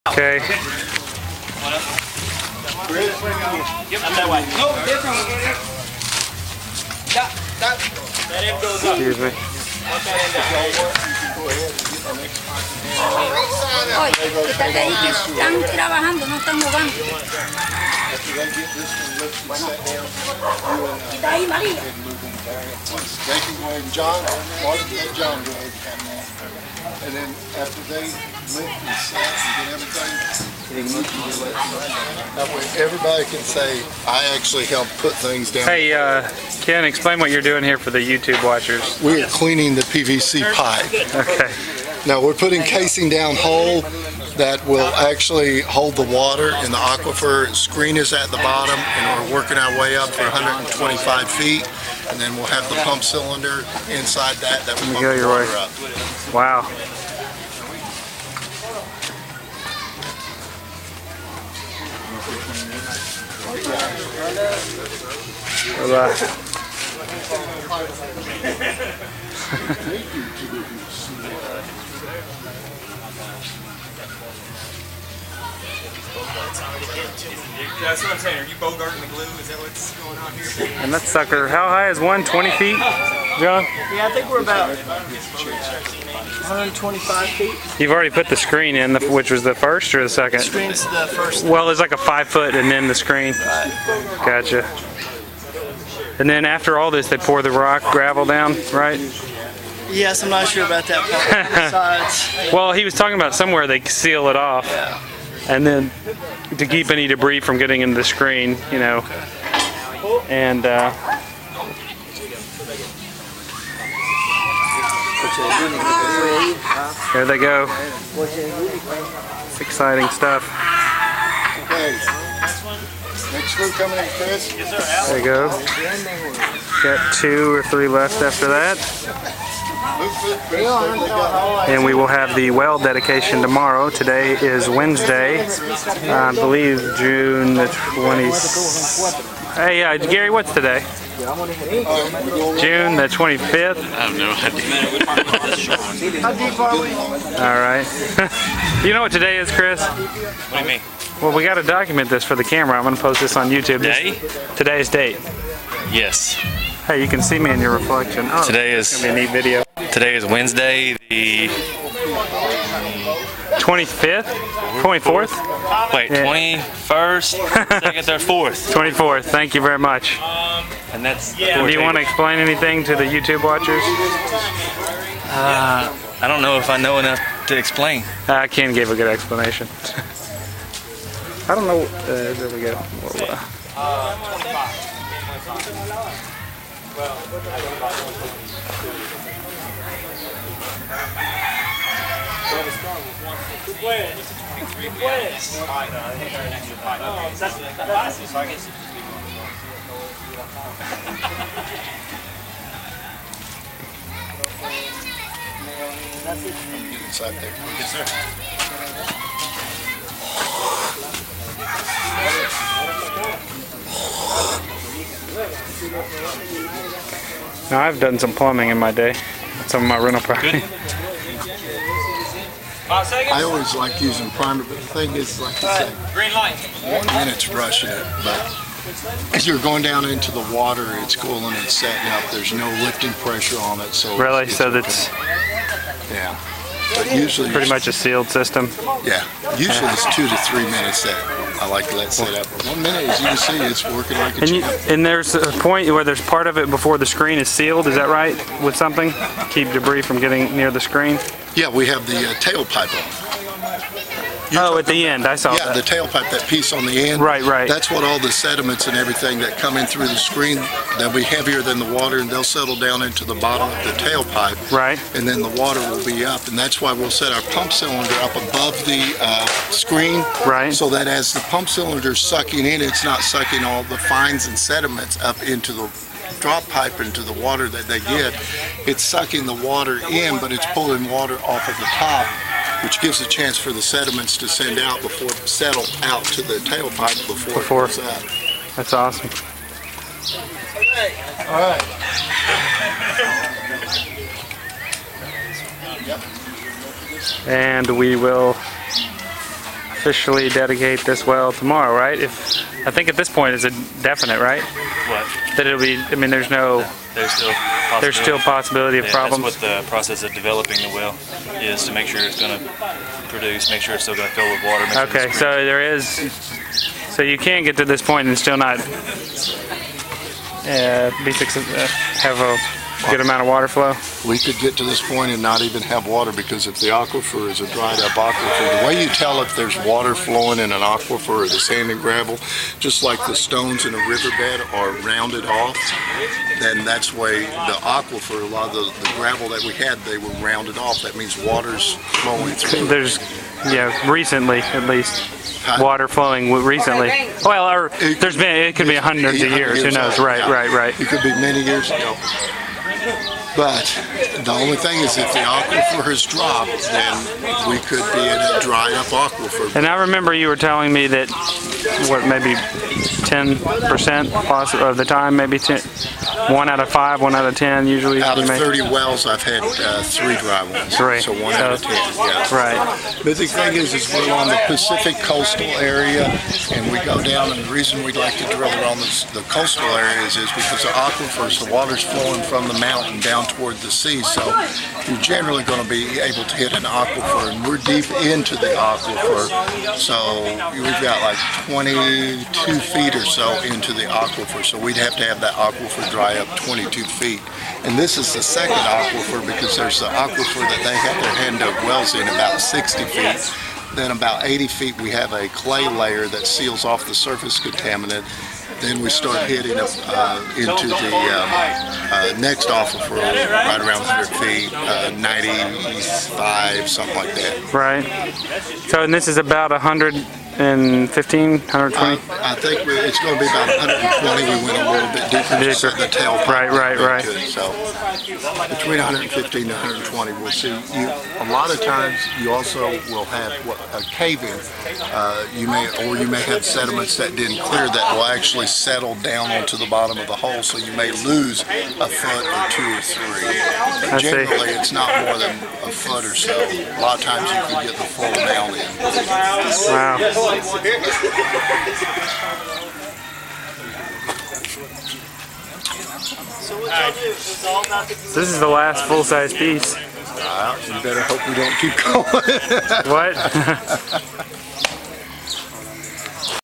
Installation of well casing, LWI Well Project: Nicaragua, 06/24/2009
This video shows the PVC casing being dropped into the newly drilled well. We drilled approximately 120 feet.. Ken explains where we were at in the process of drilling and installing the well. This video was taken on Wednesday June 24, 2009. This well project was completed (Yes, we had water!) and made possible by Living Water International and the hard-working volunteers.